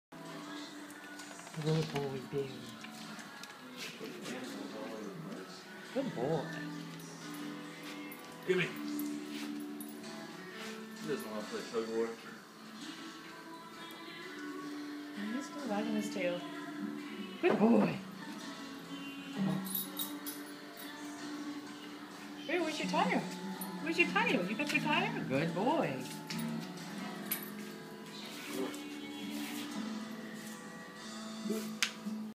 Bear Oatman, 2 days before he died of liver cancer
At least we had a few days warning, so the Nor Cal boys could come down and play with him..